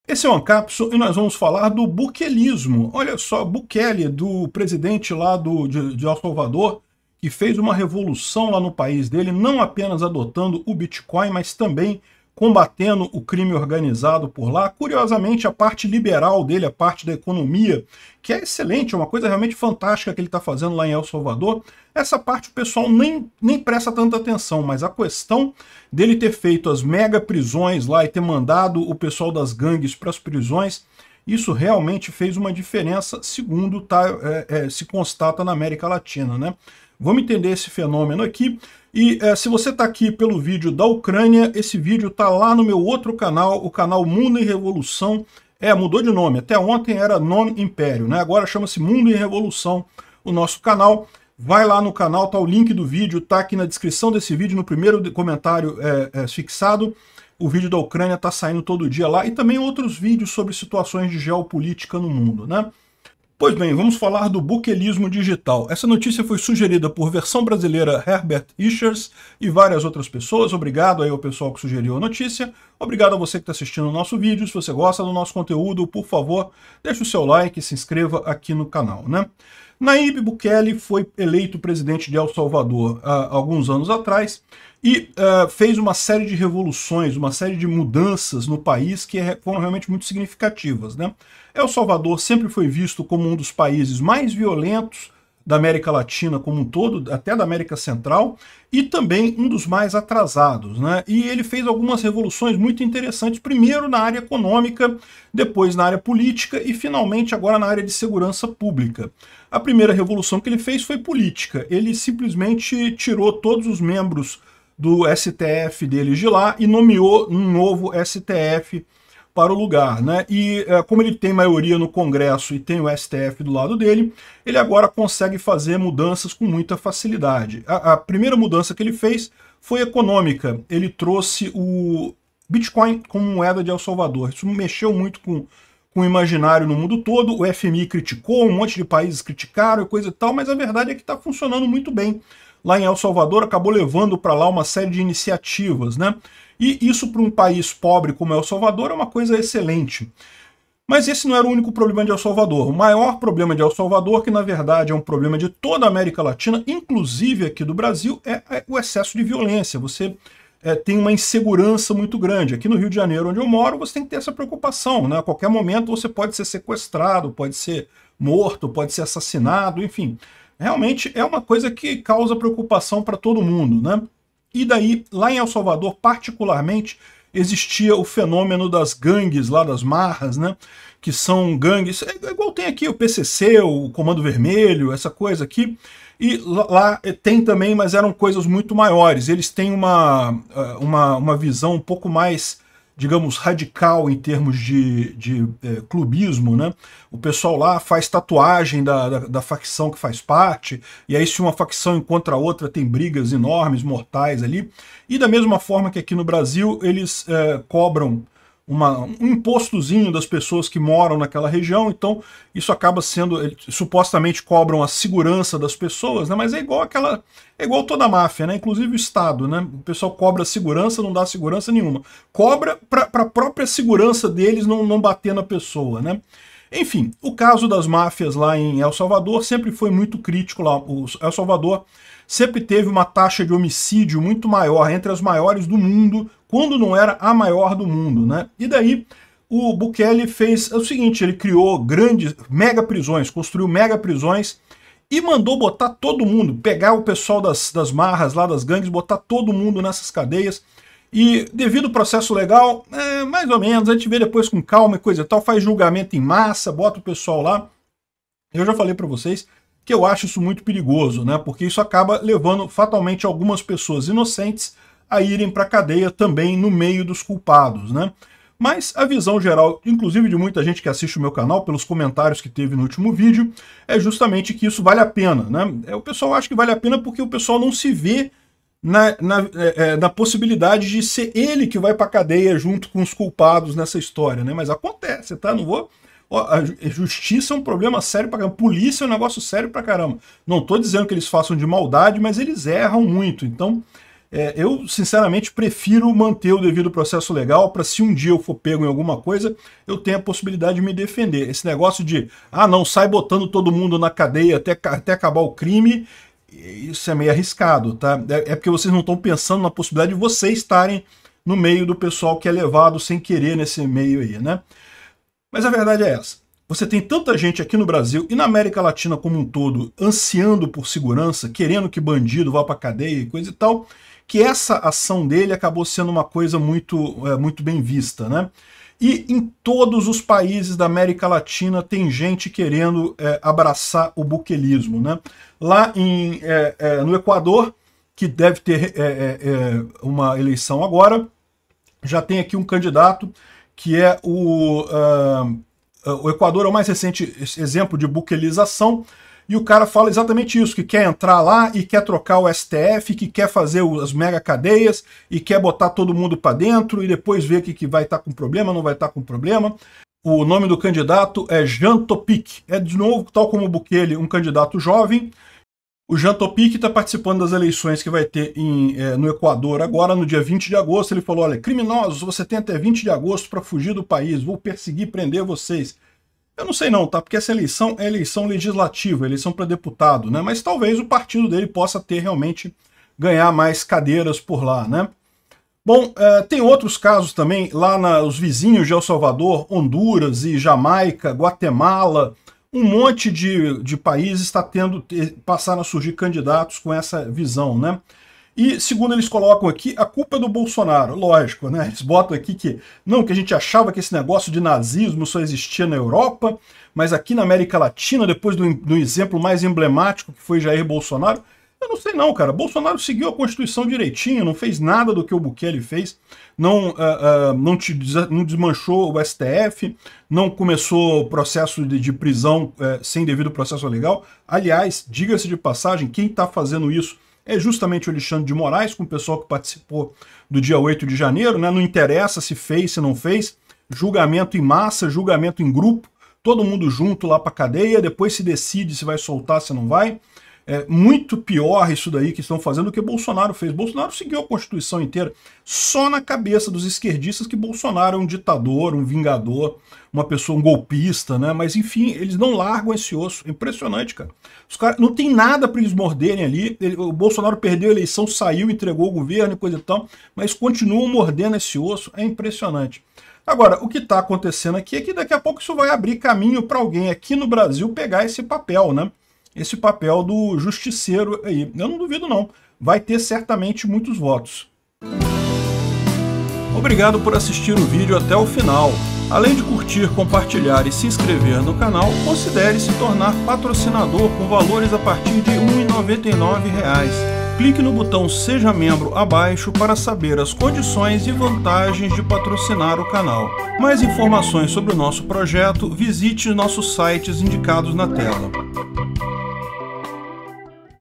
BUKELISMO se ALASTRA pela AMÉRICA LATINA com CANDIDATOS PROMETENDO SUPERPRISÕES e a culpa é de BOLSO
** APROVEITA e se INSCREVE LÁ! **

Ajude o canal:

16vmNcrA4Mvf7CaRLirAmpnjz1ZH3bWNkQ (Bitcoin)
LSCnrubCVcpuLrGTTLMqpwRTXvYz7vMbbA (Litecoin)
0x28aec946919c70e5e25d7c6785ede7622278b463 (Ethereum)
nano_1s6i6xwujzqnmie8nc3x8rfumdnebpsd6h4cp9wgkcpk4eb1xn5u7n48ok5b (Nano)
0x1302536c28695e47aaedd020aaf39f500cf6c22f (BNB)